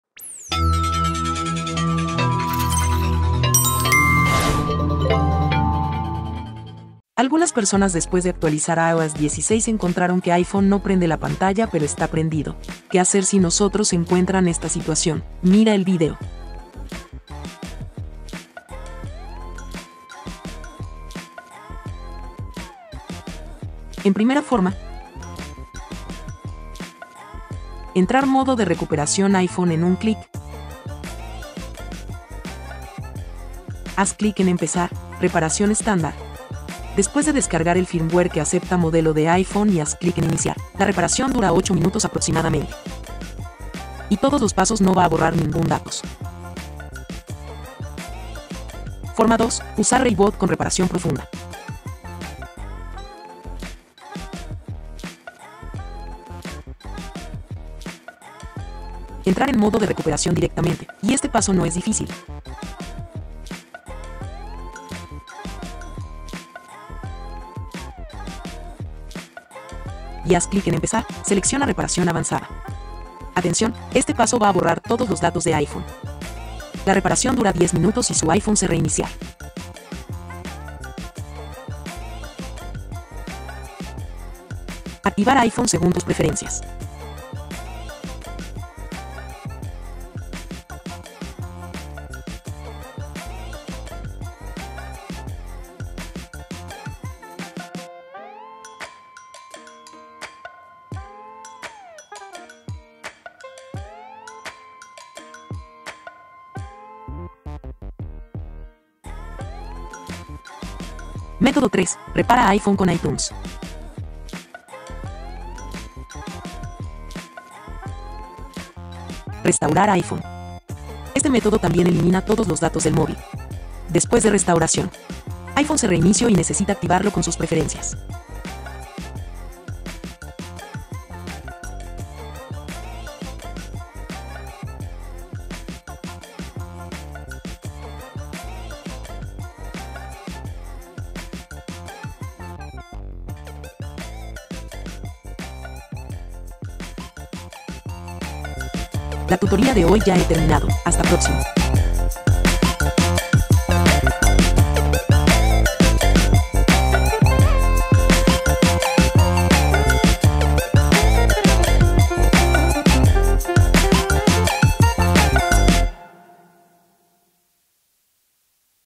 😏IPHONE NO ENCIENDE | iPhone no enciende la pantalla pero está prendido en 3 métodos😏
Algunas personas después de actualizar iOS 16 encontraron que mi iPhone no enciende la pantalla pero está prendido. qué hacer si nosotros encuentran esta situación, mira el vídeo

No sólo es un programa para restaurar dispositivos, sino también un software para realizar downgrade del software iOS, solucionar problemas de Apple TV y resolver +50 problemas del software de cualquier iPhone.

0:00 Introducción
0:16 Forma 1: reparar iPhone sin borrar nada
0:44 Forma 2: reparar iPhone sin iTunes
1:47 Forma 3: reparar iPhone con iTunes

📃 Artículo relacionado: Qué hacer si mi iphone está prendido pero no se ve la pantalla